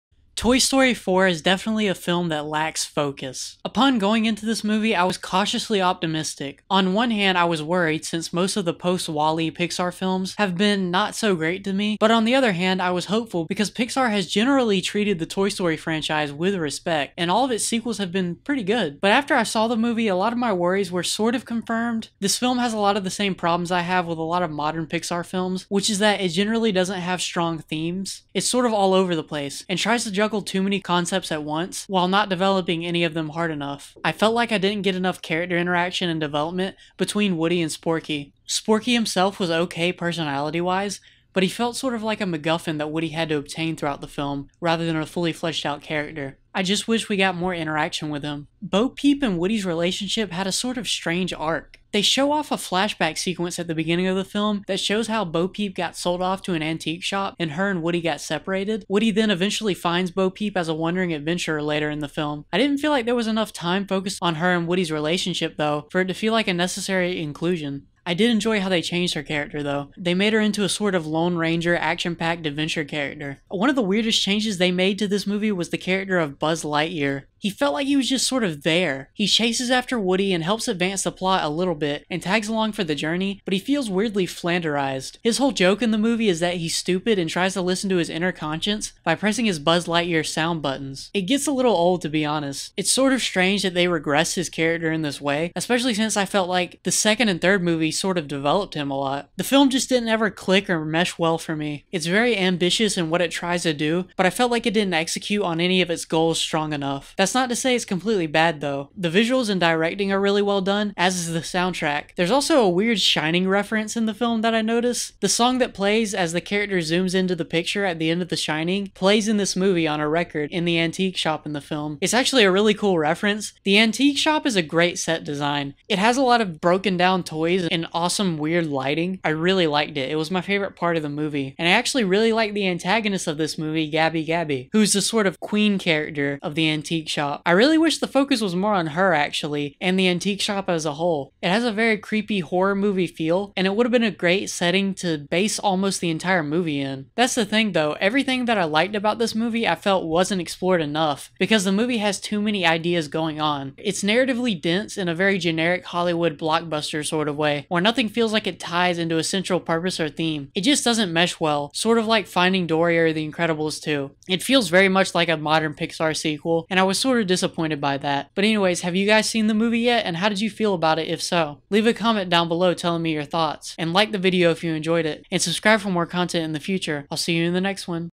Toy Story 4 - A Mixed Bag
Toy Story 4 is the latest modern Pixar film and it certainly has its ups and downs!